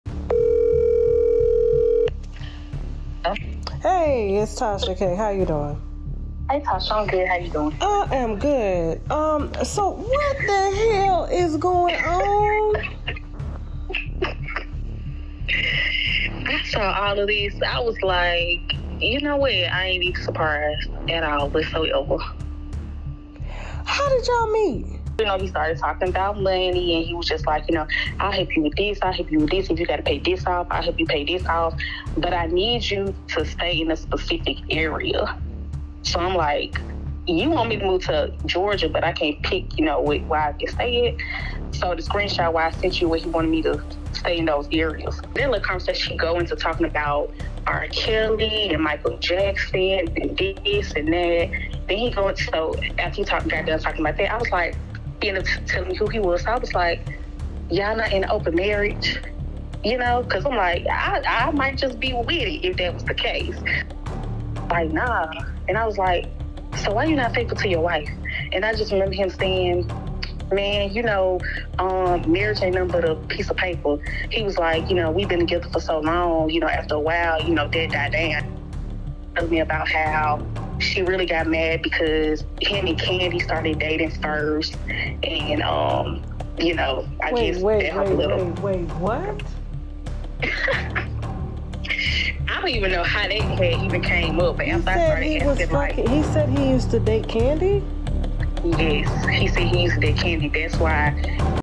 Another 1! Rocky Bivens 3rd Alleged Mistress Speaks! (Premiers Tonight at 9pm est on TashaKLive.com)
Rocky Bivens 3rd Mistress (Premiers Tonight at 9pm est on TashaKLive.com)